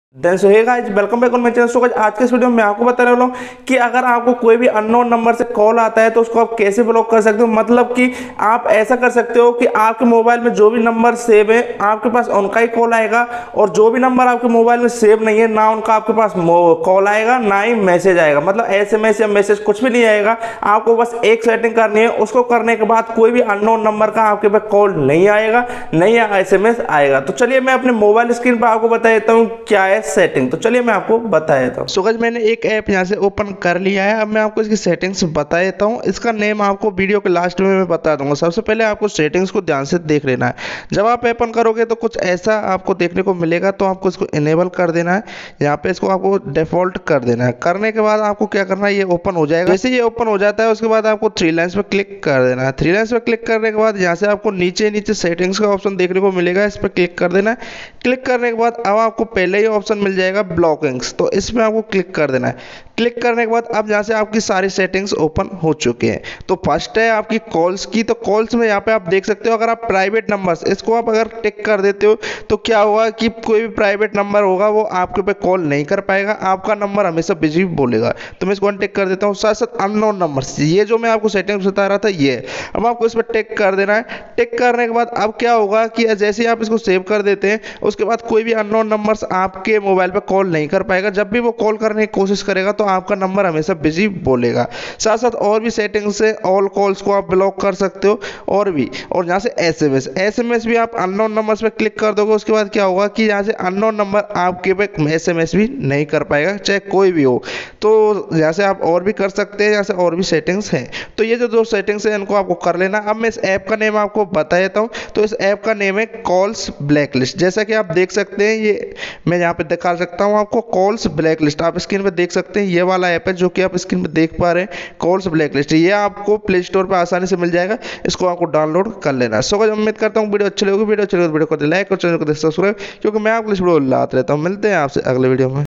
How to block unknown numbers | Unknown number call block app | Block unknown number and SMS
About video
Dosto Is video me maine btaya h How to block unknown Calls & Sms both in Android smartphone.

Vinod techno